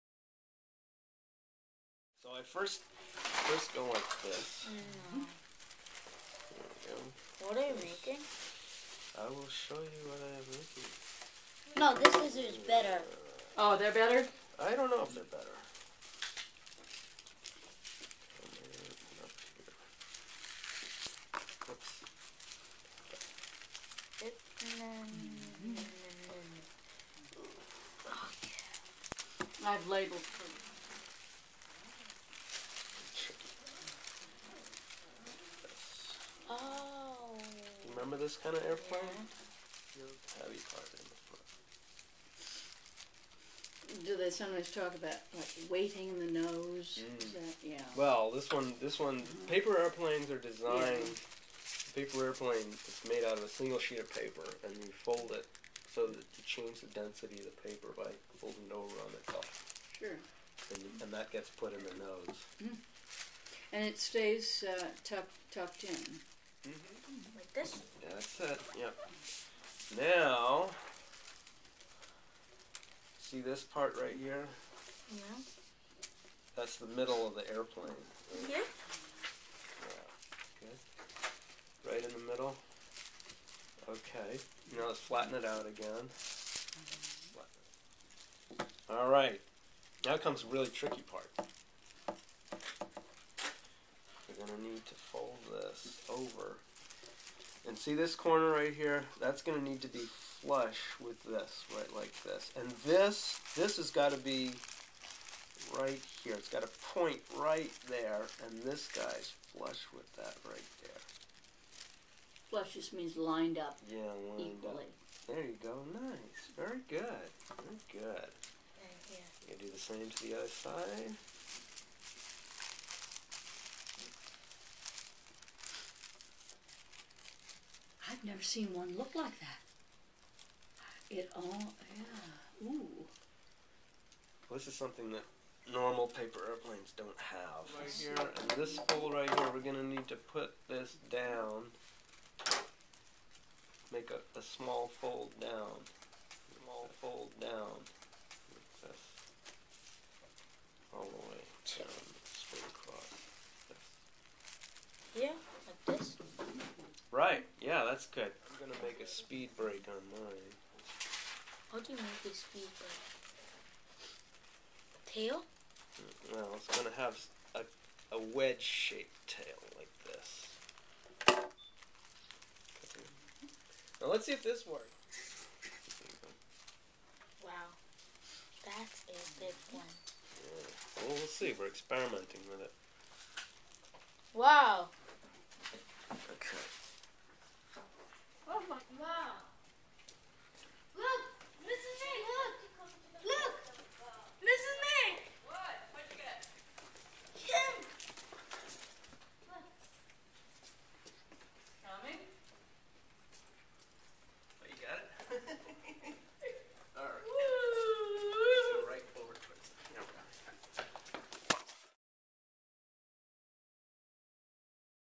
Mrs. Nic's Class Makes and Flies the Surfer Paper Airplane
Mrs. Nic's class makes the tumblewing and paper airplane surfer walkalong gliders. For more information on making and flying the tumblewing and paper airplane surfer walkalong gliders see: